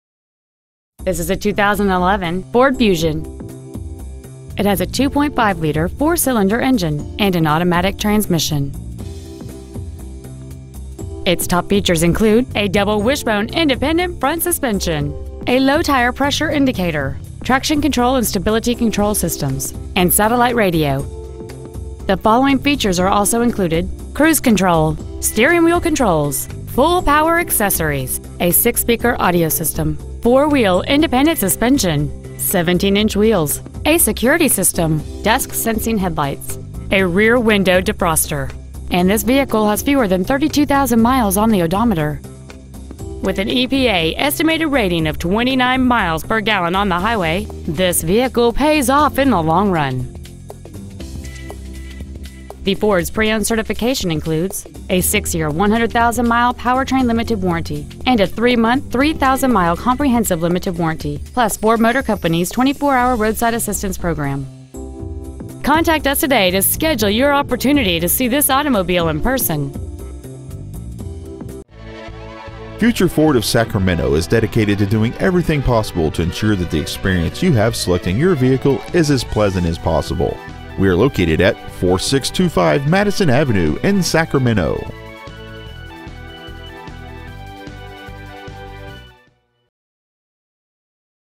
2011 Ford Fusion Certified Sacramento CA
Year : 2011
 Make : Ford
 Model : Fusion
 Engine : 2.5L I4 16V MPFI DOHC
 Trans . : AUTOMATIC
 Exterior : WHITE SUEDE
 Miles : 31,041

 Stock : R82657